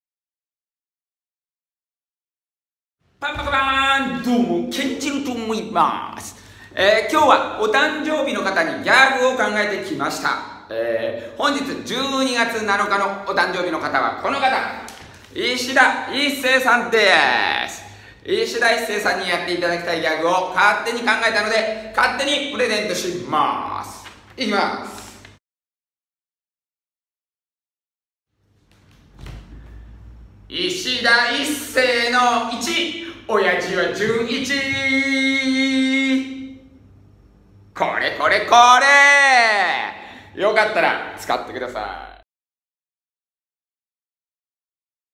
12月7日のお誕生日ギャグ いしだ壱成さん
いしだ壱成さんのギャグを勝手に考えたので勝手にプレゼントしまぁ〜す